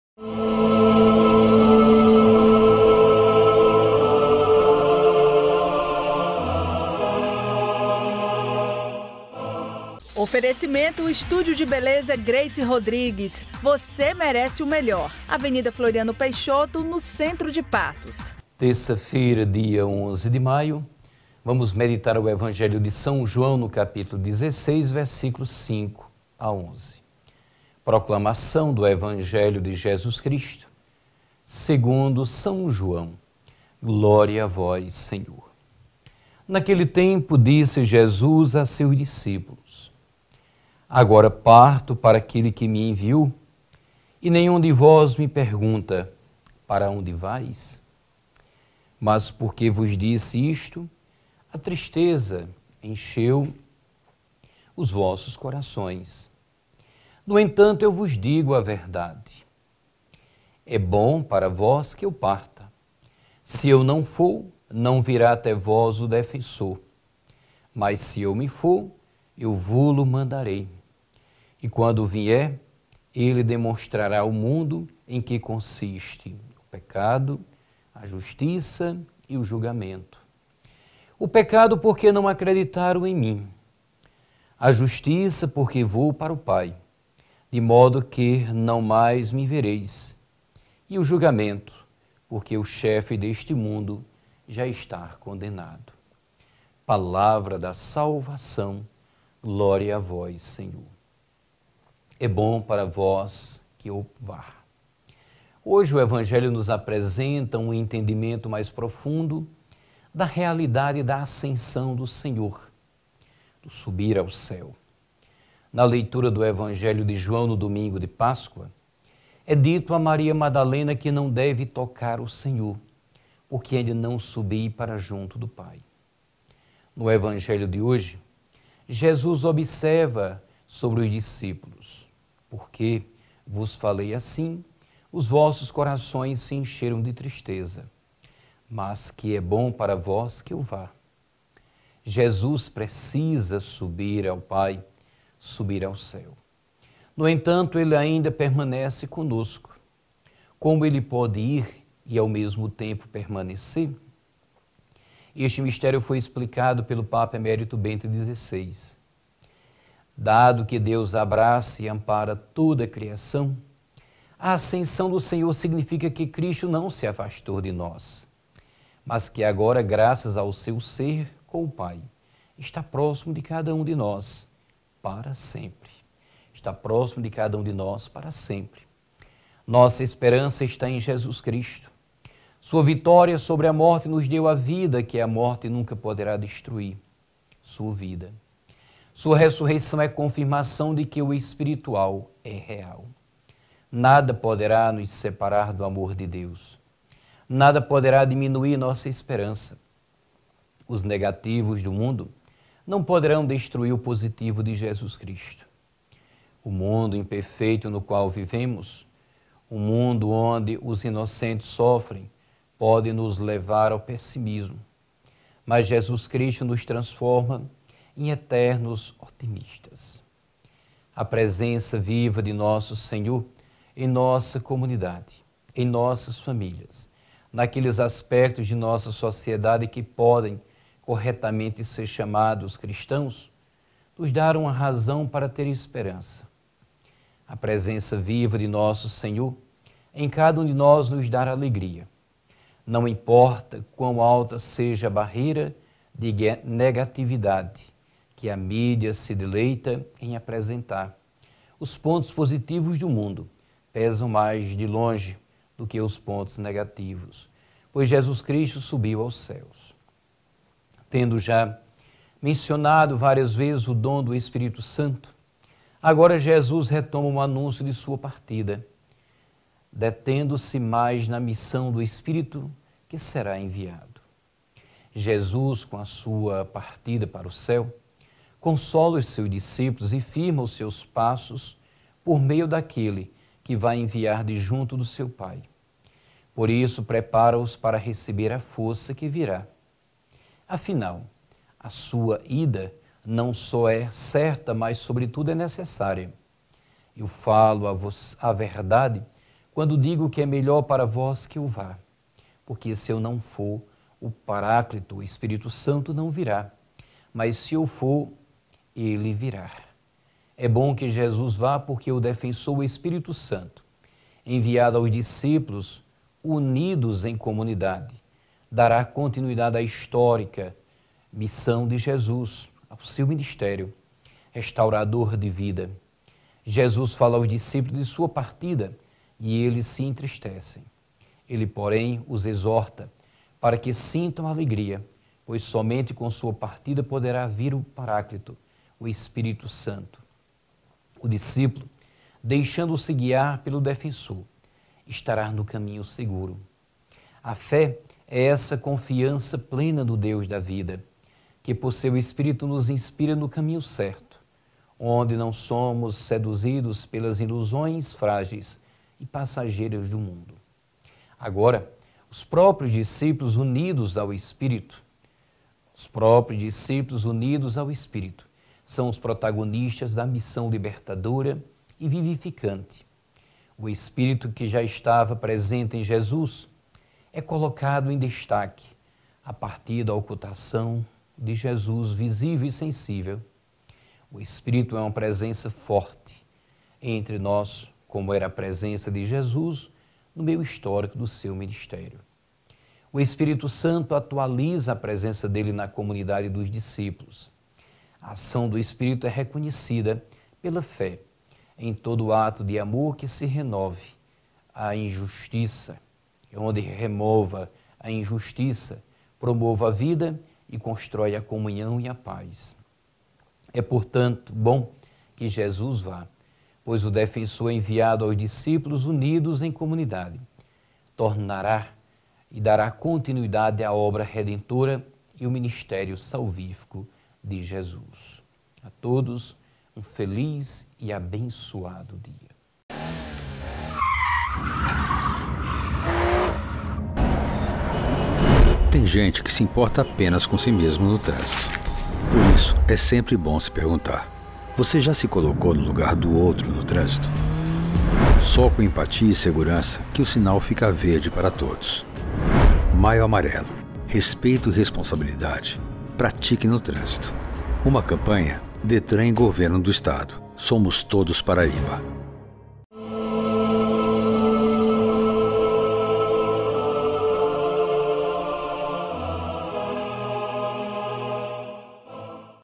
EVANGELHO DO DIA | JOÃO 16, 5-11
Comece bem a sua terça-feira (11/05/2021) ouvindo a palavra de Deus. Na manhã de hoje, Padre Joácio Nóbrega traz a reflexão do Evangelho de João 16, 5-11.